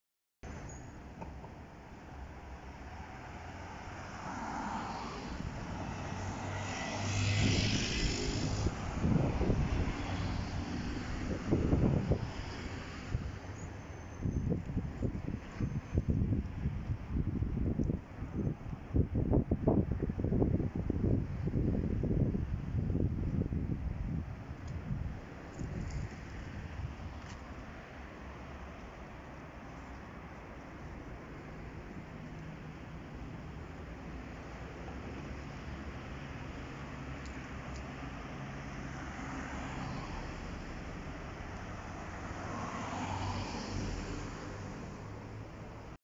Av. Santa Julia En La Tarde
Pocos vehículos particulares pasan en avenida Santa Julia y frente de la parroquia.

Cuídense amigos, ya en fase 4.